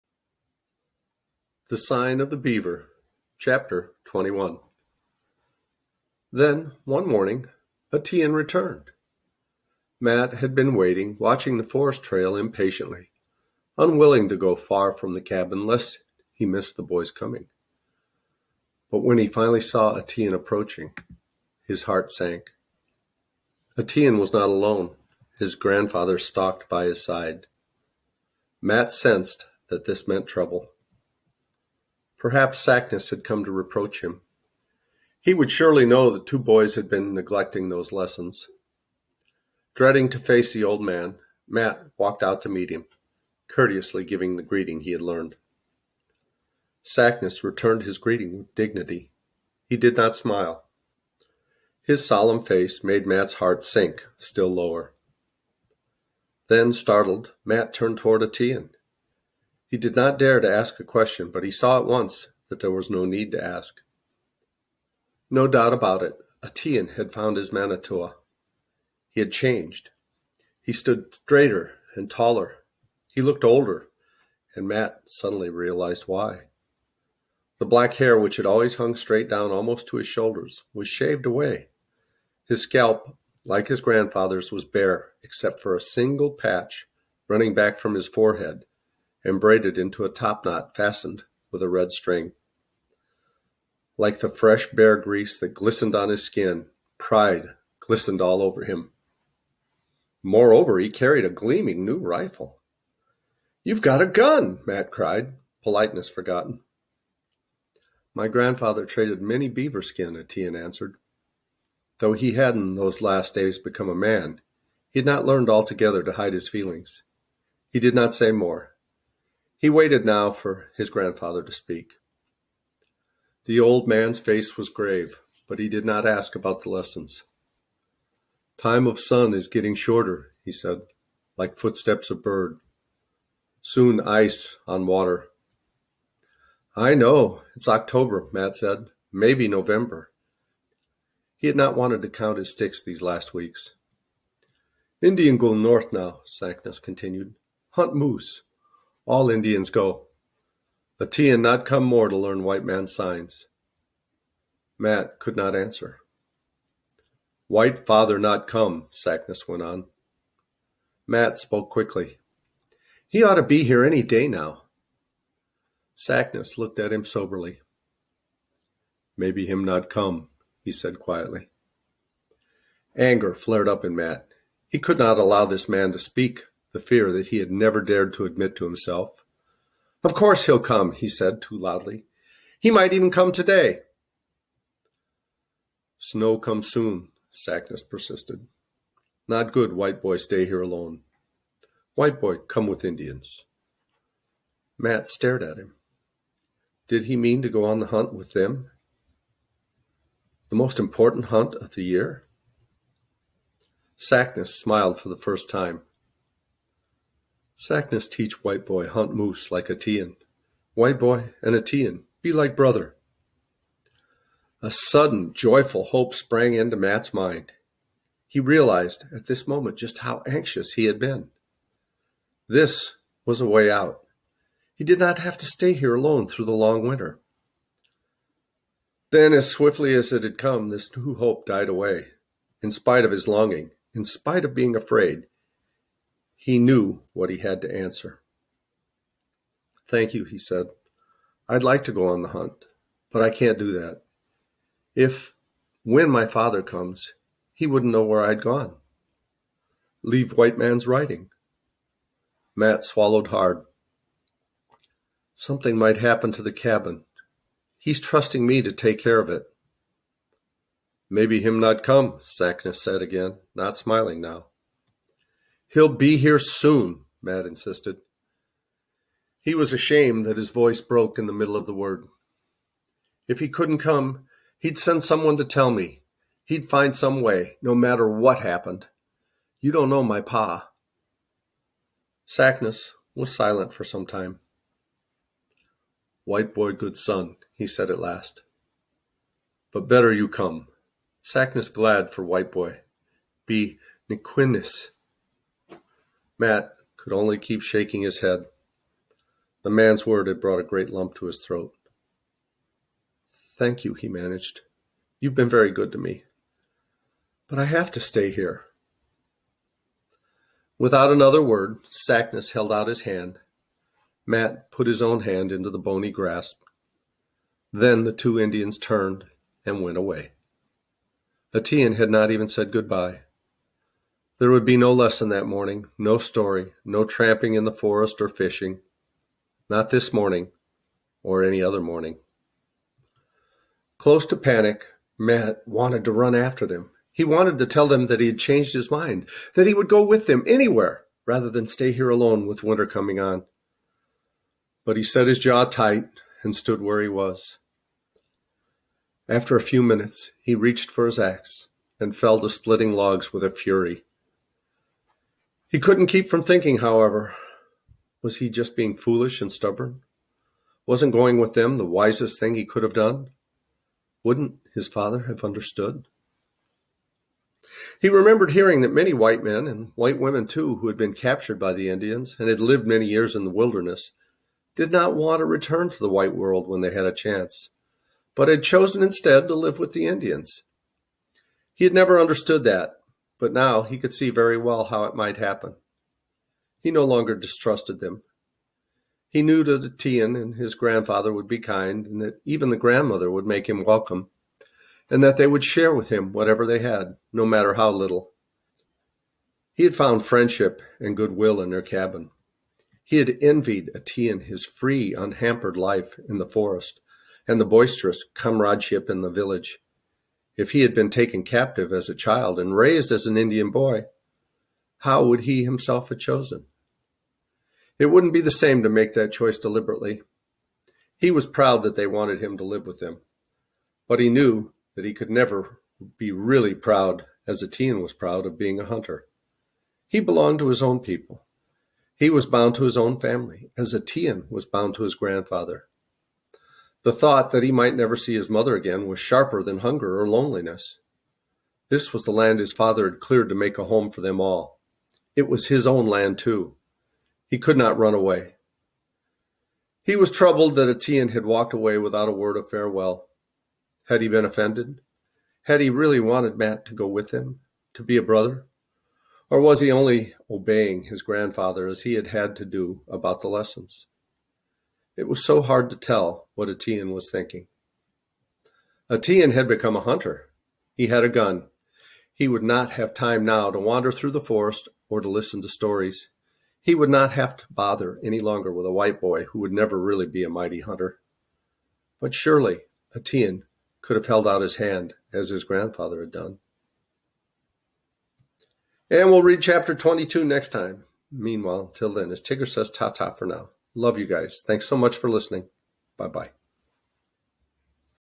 The sign of the beaver chapter 21 | CC Challenge 1| audio book | Elizabeth George Speare
"The Sign of the Beaver" is a CC Challenge 1 audio book by Elizabeth George Speare.